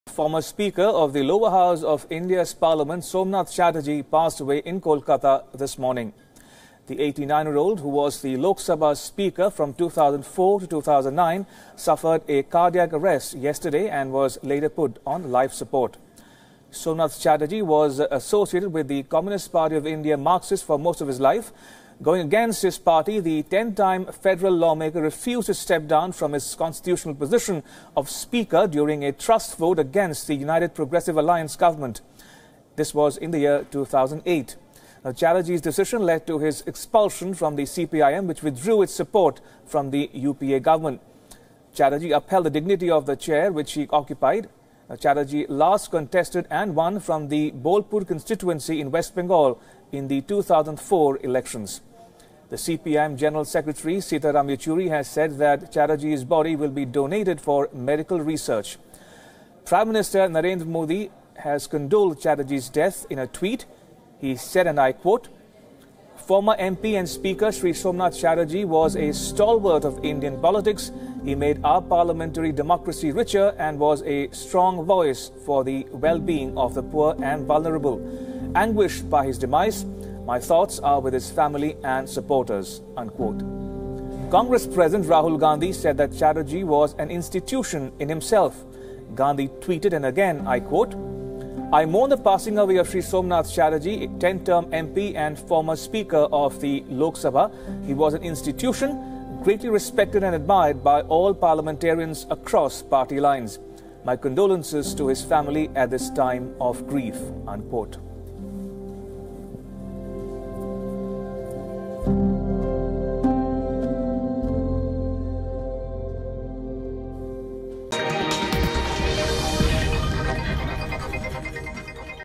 Former Lok Sabha speaker Somnath Chatterjee dies at 89
Former Lok Sabha speaker Somnath Chatterjee died early Monday morning in Kolkata. He was 89.